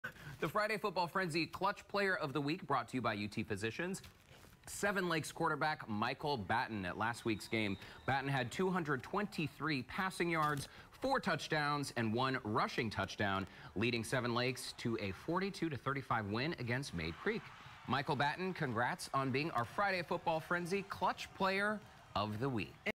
Michael Batton named Clutch Player of the Week
The Seven Lakes quarterback helped lead his team to victory